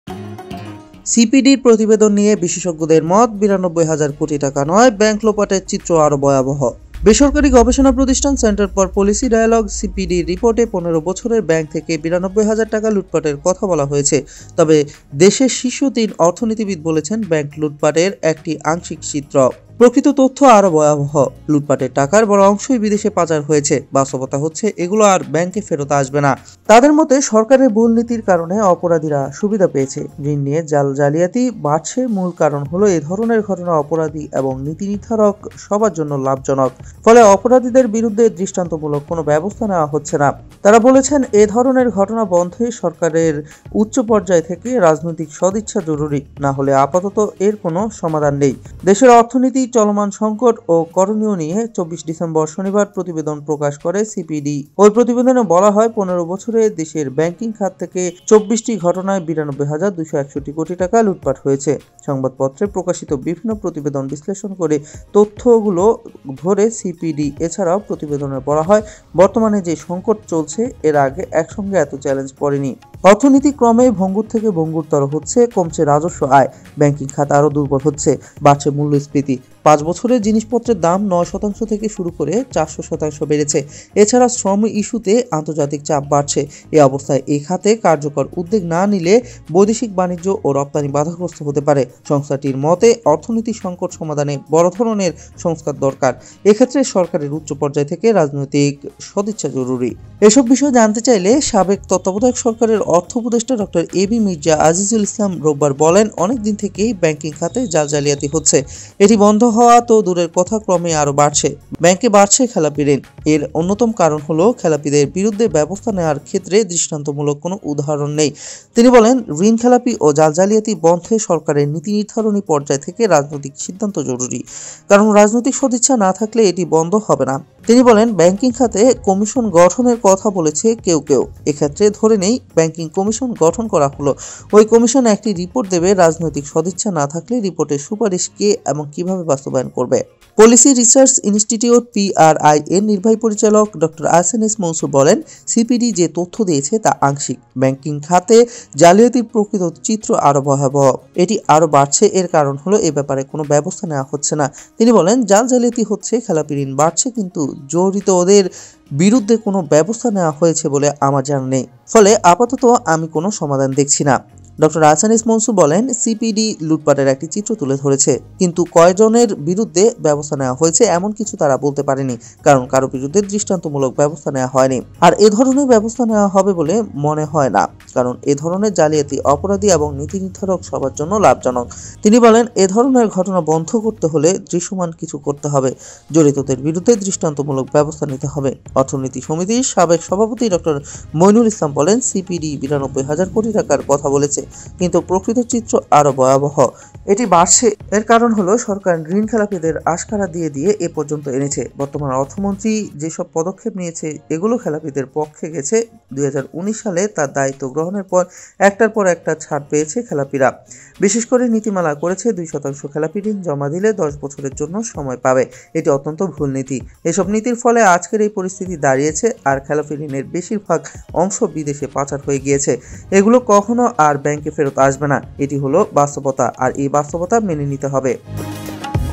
সিপিডির প্রতিবেদন নিয়ে বিশেষজ্ঞদের মত ৯২ হাজার কোটি টাকা নয়- ব্যাংক লোপাটের চিত্র আরও ভয়াবহ
সিপিডির প্রতিবেদন নিয়ে বিশেষজ্ঞদের মত
৯২ হাজার কোটি টাকা নয়- ব্যাংক লোপাটের চিত্র আরও ভয়াবহ
বেসরকারি গবেষণা প্রতিষ্ঠান সেন্টার ফর পলিসি ডায়ালগ-সিপিডির রিপোর্টে ১৫ বছরে ব্যাংক থেকে ৯২ হাজার টাকা লুটপাটের কথা বলা হয়েছে। তবে দেশের শীর্ষ তিন অর্থনীতিবিদ বলেছেন ব্যাংক লুটপাটের এটি আংশিক চিত্র। প্রকৃত তথ্য আরও ভয়াবহ। লুটপাটের টাকার বড় অংশই বিদেশে পাচার হয়েছে। বাস্তবতা হচ্ছে, এগুলো আর ব্যাংকে ফেরত আসবে না।

তাদের মতে সরকারের ভুল নীতির কারণে অপরাধীরা সুবিধা পেয়েছে। ঋণ নিয়ে জাল-জালিয়াতি বাড়ছে। মূল কারণ হলো এ ধরনের ঘটনা অপরাধী এবং নীতিনির্ধারক সবার জন্য লাভজনক। ফলে অপরাধীদের বিরুদ্ধে দৃষ্টান্তমূলক কোনো ব্যবস্থা নেওয়া হচ্ছে না। তারা বলছেন, এ ধরনের ঘটনা বন্ধে সরকারের উচ্চপর্যায় থেকে রাজনৈতিক সদিচ্ছা জরুরি, না হলে আপাতত এর কোনো সমাধান নেই।